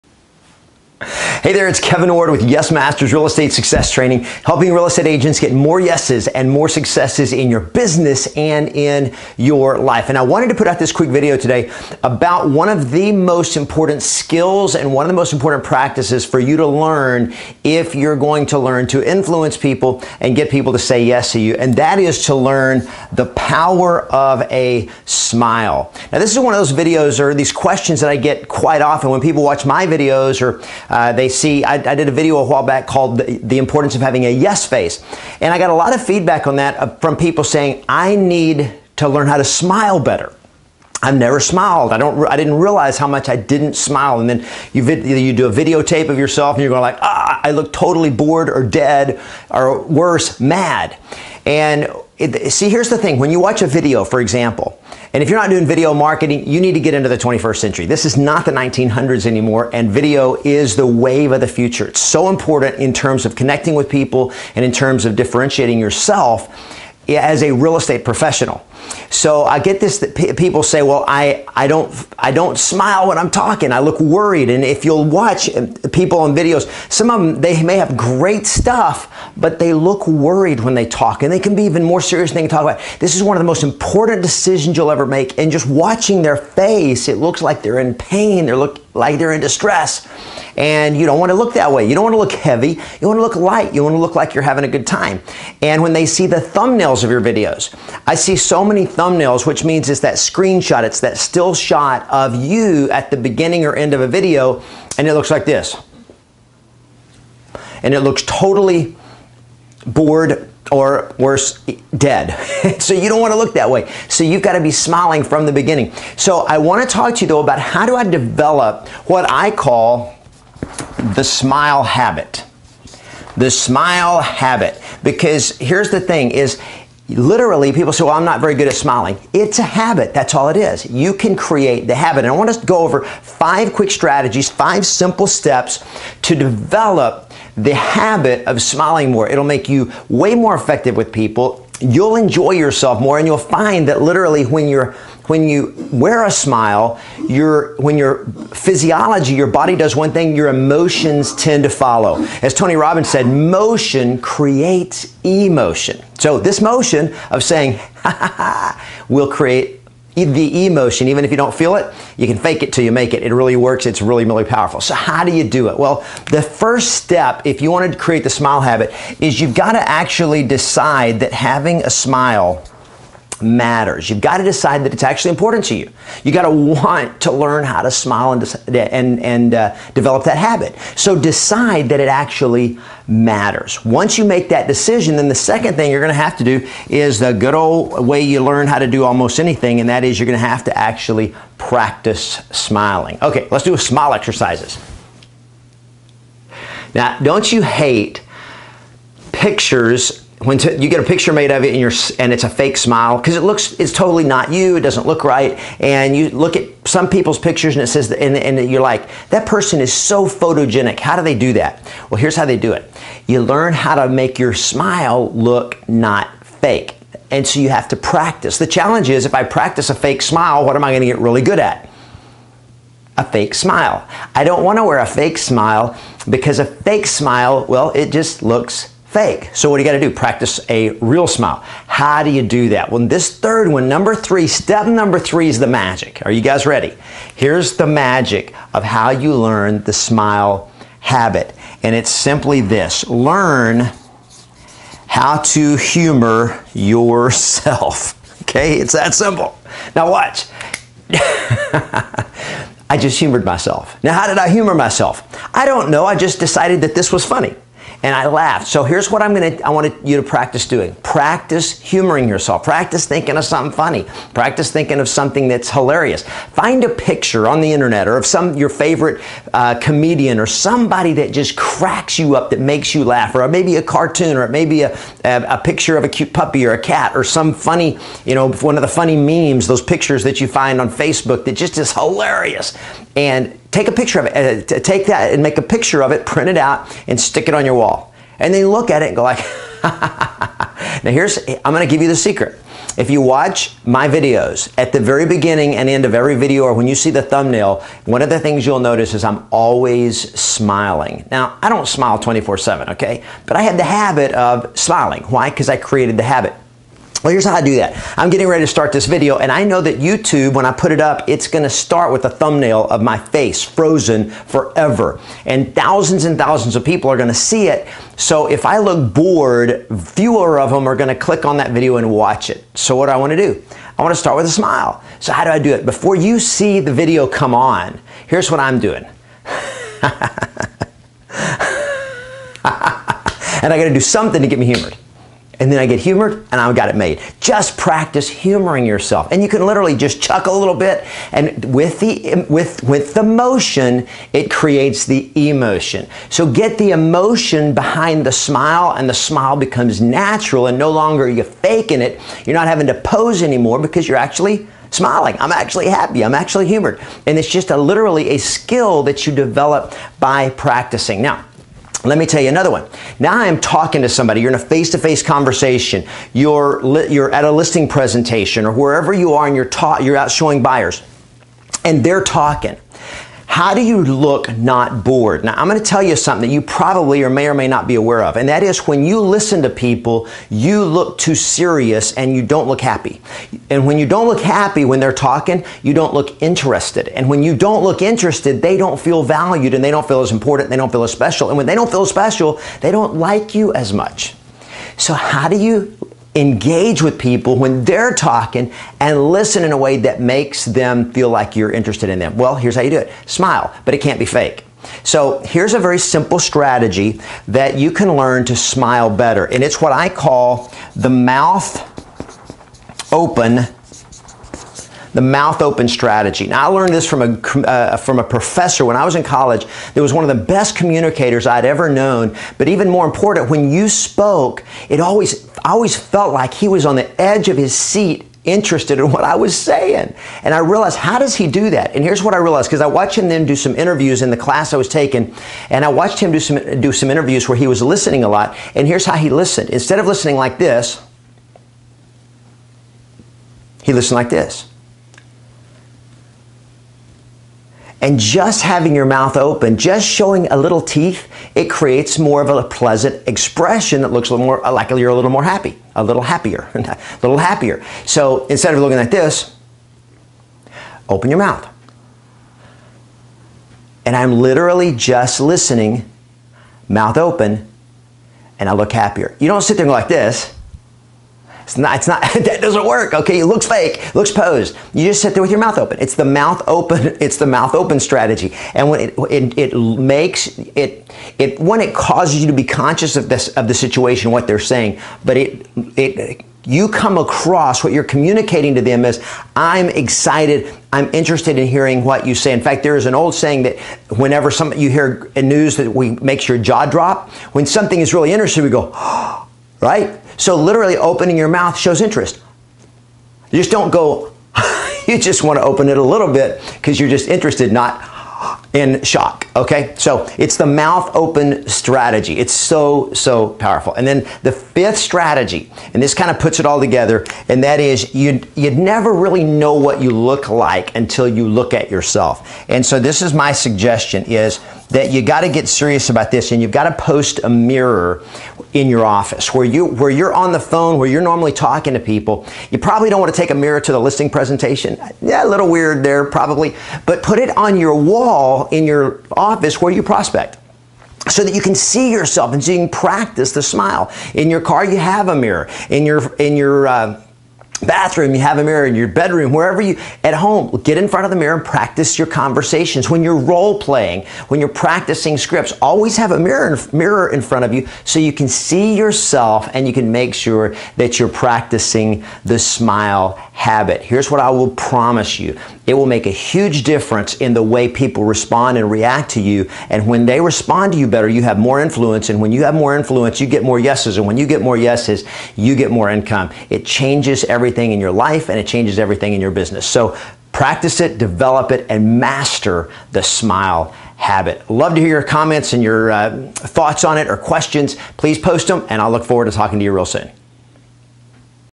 Get More Listings by Learning How to Smile - Kevin Ward @ YesMasters.com
In this over the edge video, real estate speaker, trainer, and coach, Kevin Ward explains the power of having a great smile.  Learning to smile is simply developing a habit of smiling (and it helps if happiness is attached!)

Kevin shares 5 Simple Steps for Creating a Genuine Smile:
1. Decide to smile more.  This really only works if you know that it matters and actually want to smile more.  It's hard to be happy against your will.
2. Practice it.
3. Humor yourself.  Learn to make yourself laugh and smile at will.  This will eliminate the fake "pasty"-looking smile.
4. Open your mouth slightly when listening to others.
5. Hang a Mirror in front of where you prospect and role play.